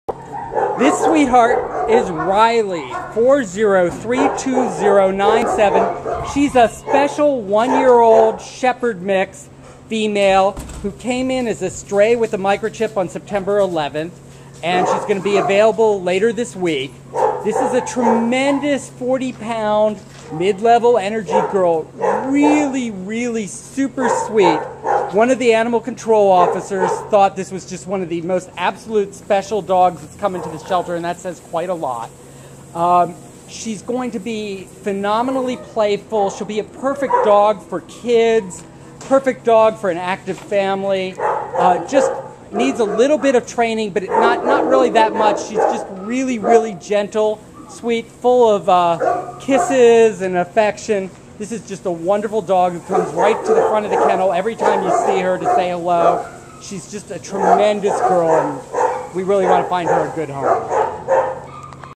Riley 4032097
Riley is a tremendous one year old tan and white female Shepherd mix puppy who came into the Baldwin Park shelter as a stray on September 11th.  The animal control officer who brought her in said she was the best, most gentle and affectionate, girl she has brought in... and we agree.  This sixty pound mid level energy girl has been well socialized and walks well on the leash.  She is extremely loving and enjoys nothing more than hanging out and being petted.  Playful and smart she will make a wonderful pet for a family living in a private home.